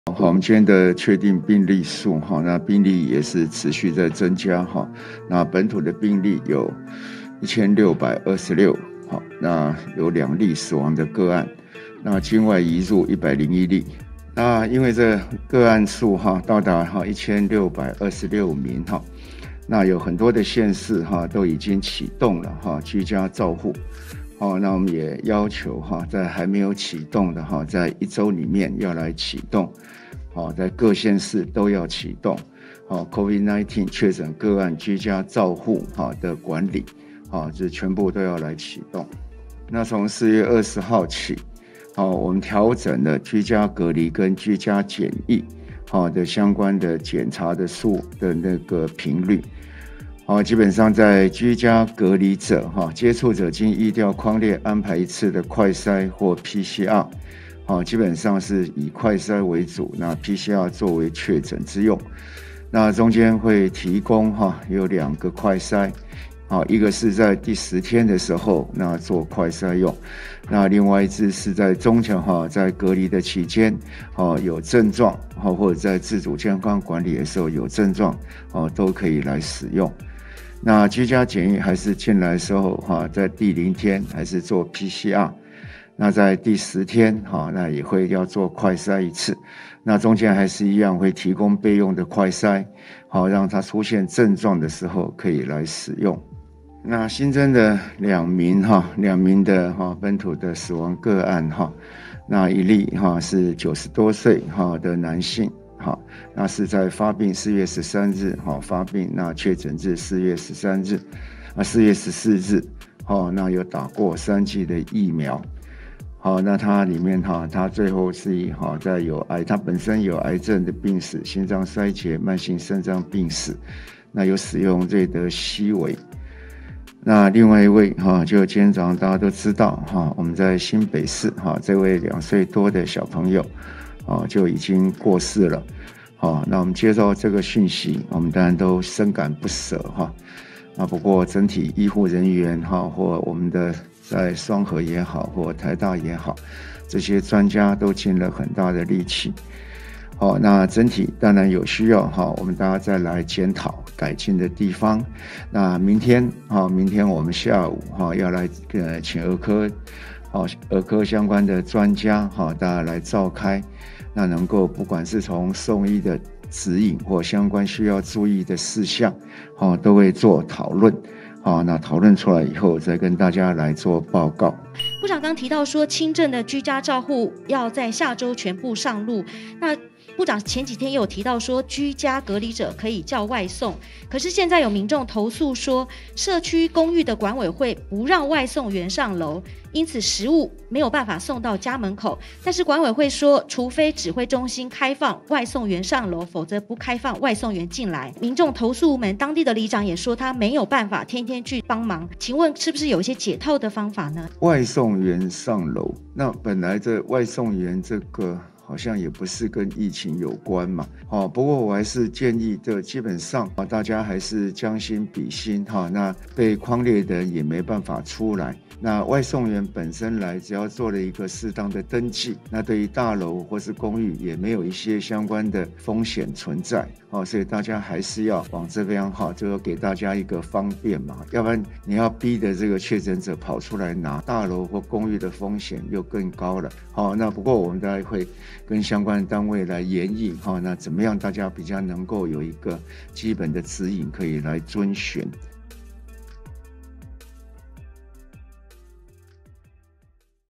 本土+1626 、增2死亡個案 本土連日破千 陳時中：全國一週內啟動居家照護
本土+1626 、境外移入+101 增2死亡個案

中央流行疫情指揮中心指揮官陳時中今日公布，本土武漢肺炎（新型冠狀病毒病，COVID-19）病例新增1626例，再度創下單日新高，個案遍及21縣市，今年原本零確診的連江縣也出現個案，但全台仍以新北市583例最多；另境外移入新增101例，其中89例為落地採檢陽性，另確診個案中新增2例死亡。